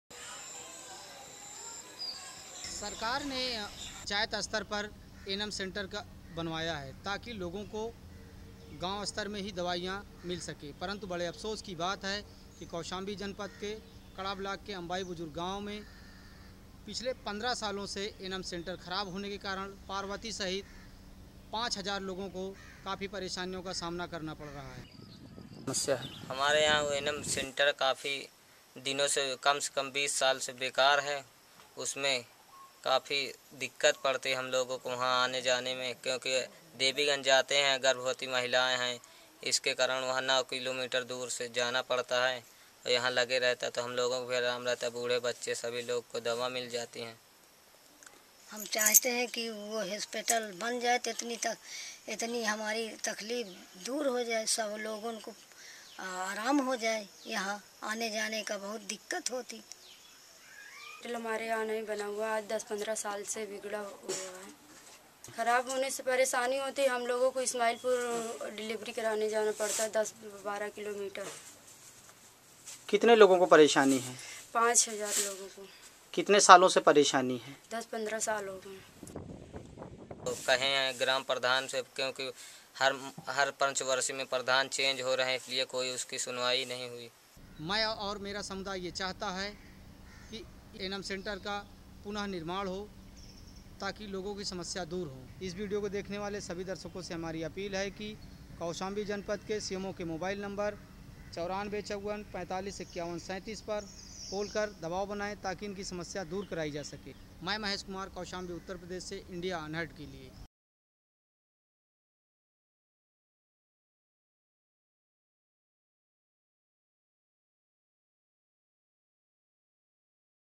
5000 people suffer due to half constructed health centre
Community Correspondent Mahesh Kumar reports from Kausambi,   Uttar Pradesh for Video Volunteers.